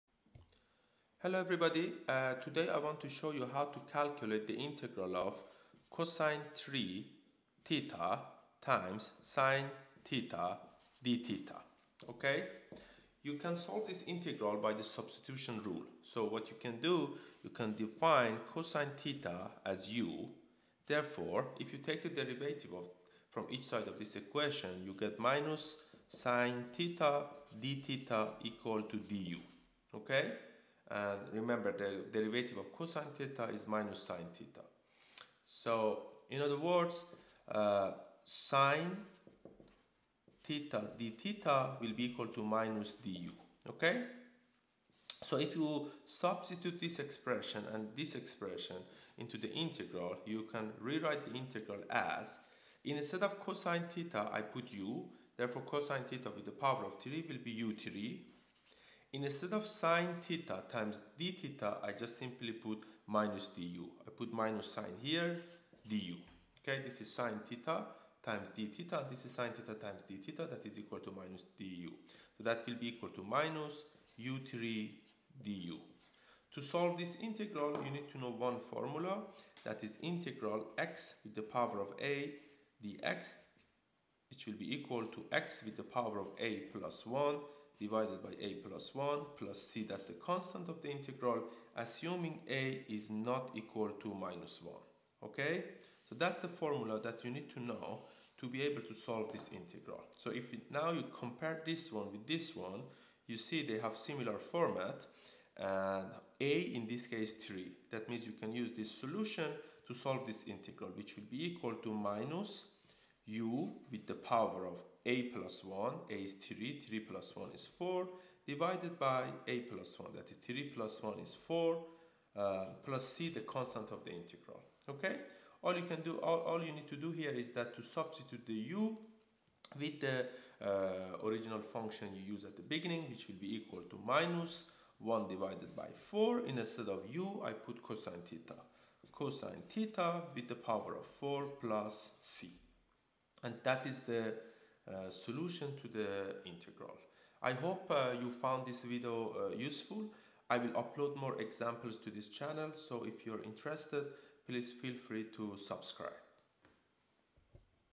Integral of cos^3(x)sin(x) - Integral example
This video shows how to find the integral of cos^3(x)sin(x).